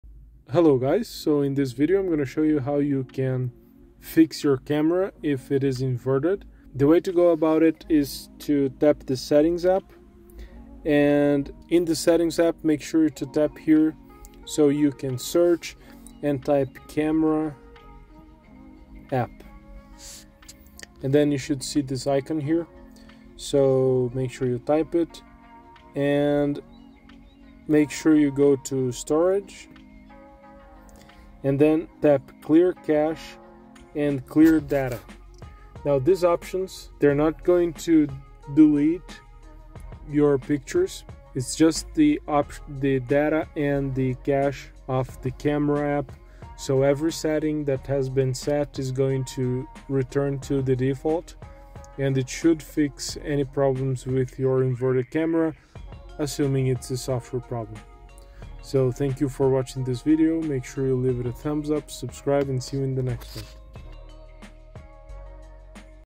How to Turn Off Inverted Camera on Android (2025)
In this video I'll show you How to Turn Off Inverted Camera on Android